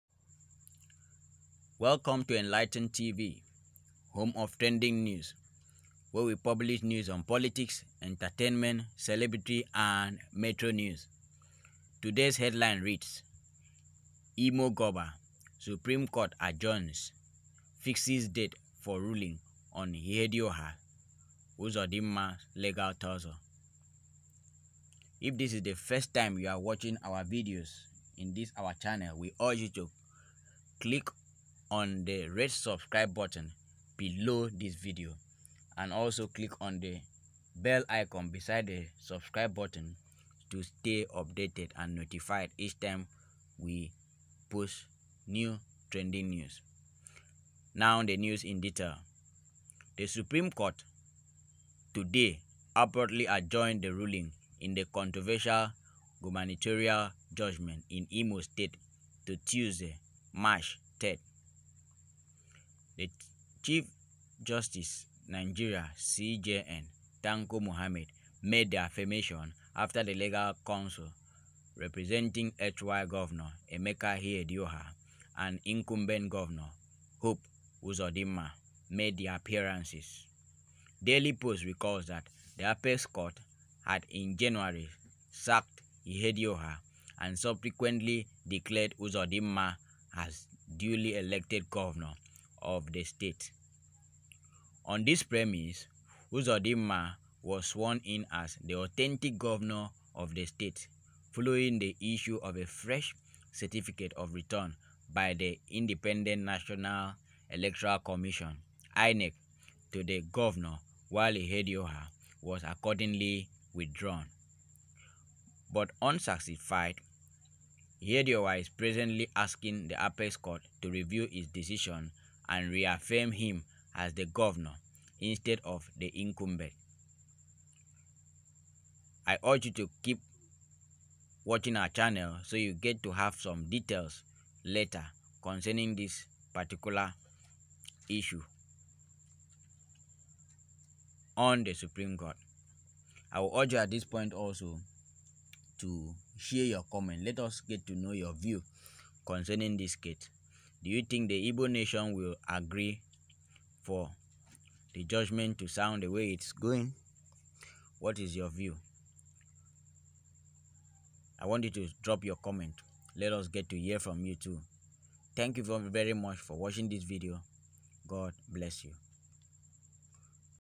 Imo Guber: Supreme Court adjourns, fixes date for ruling on Ihedioha, Uzodinma’s legal tussle
The Supreme Court, Monday abruptly adjourned the ruling in the controversial gubernatorial judgement in Imo state to Tuesday, March, 3.